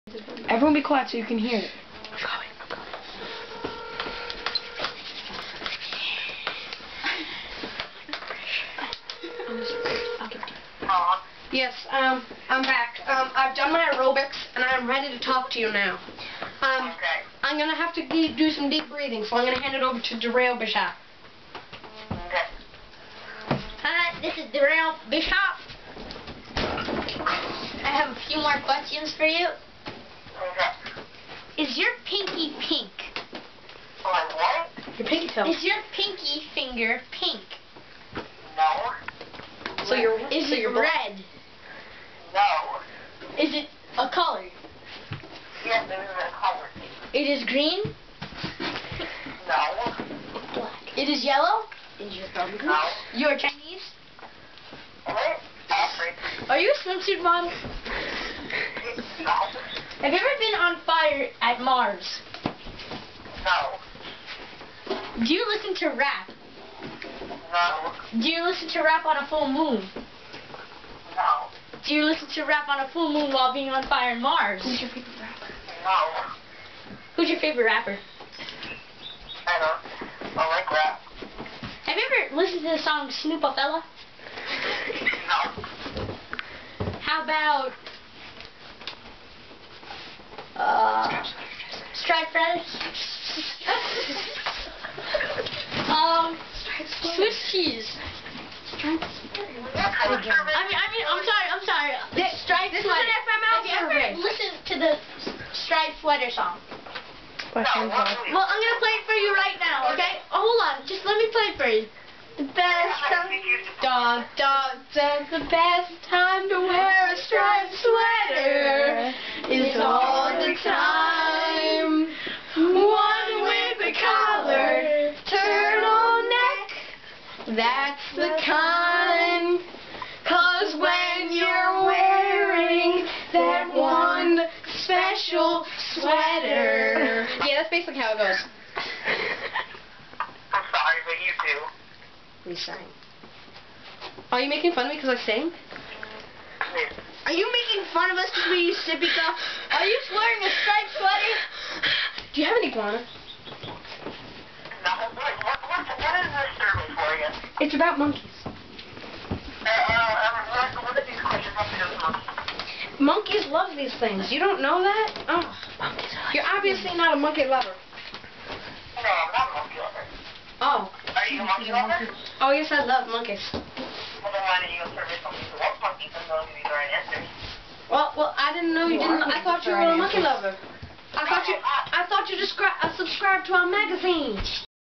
prank calls part 2
at patricks house again.doing prank calls part two..
ben dover-patrick
mike rotch-stevn
derial bishop-mahdi
video taping by sam.